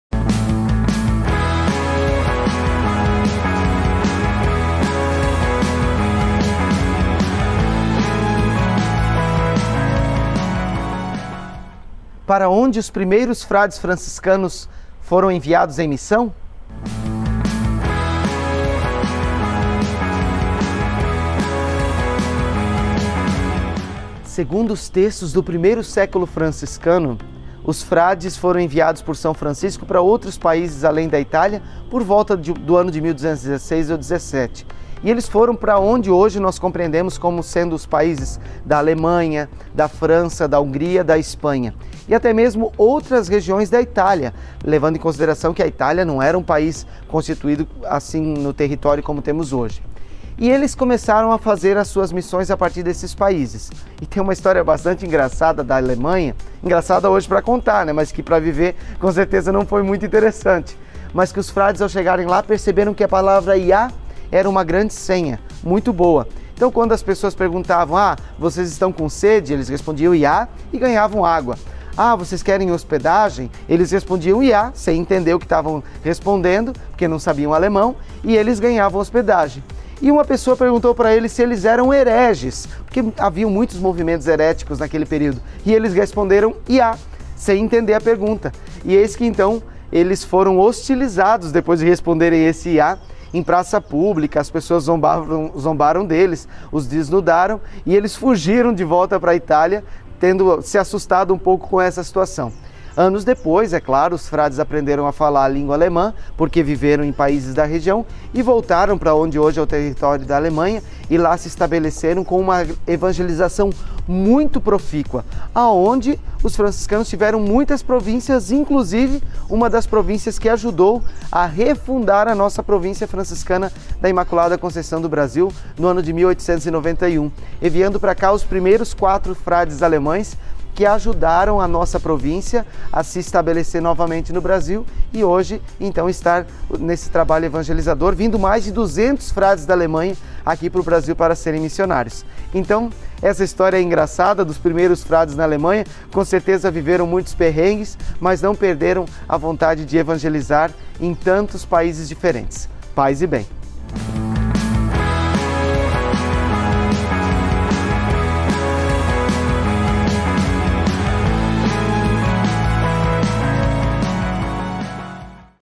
serfranciscano - Para onde os primeiros frades foram enviados em missão?
Hoje o Frei Gabriel Dellandrea,Ofm  vai falar sobre este significado.
Acompanhe, curta e compartilhe!